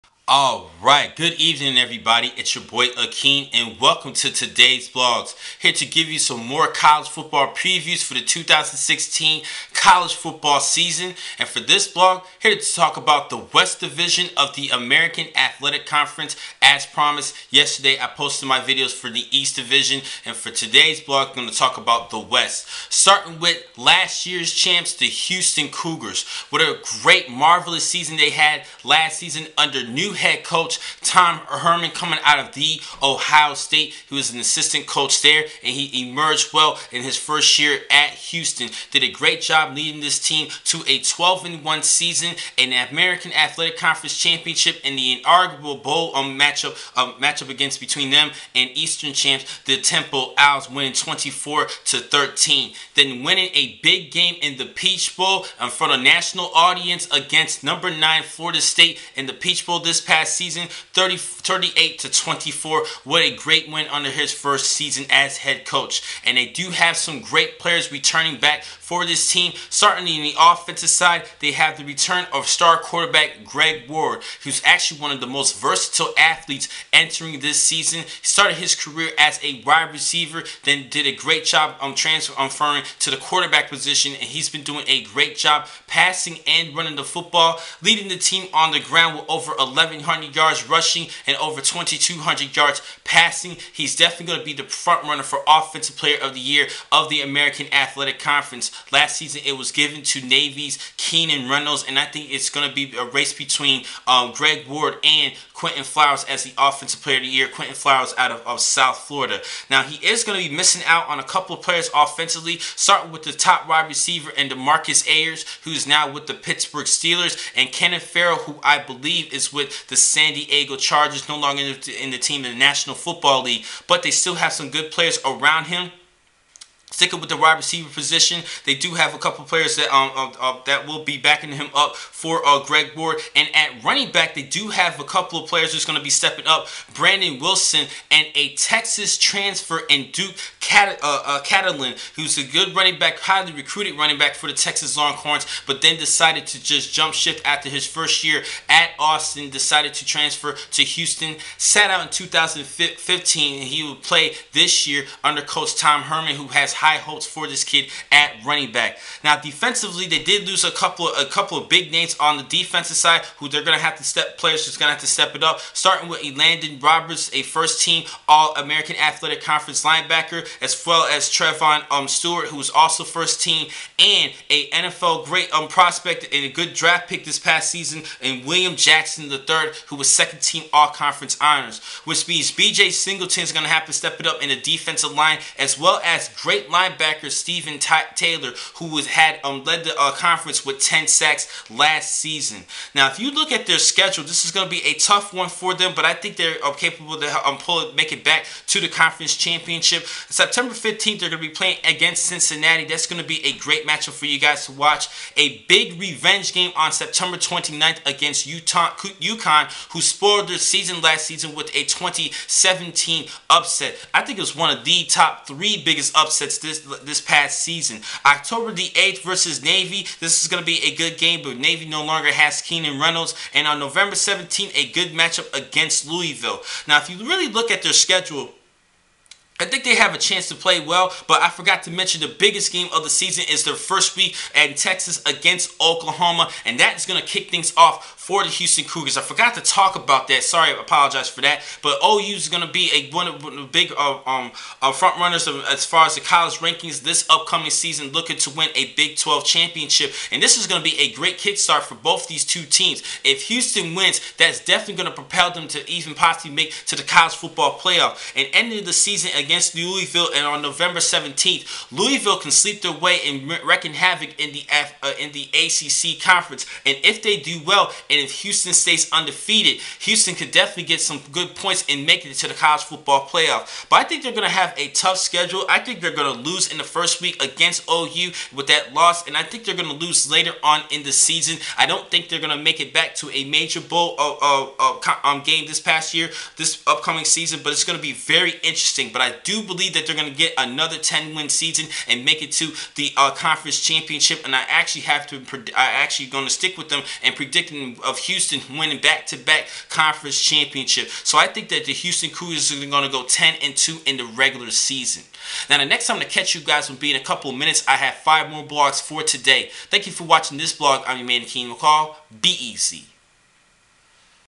Houston Cougars Preview: 2016 College Football Season
My quick analysis and prediction of the Temple Owls for the 2016 college football season.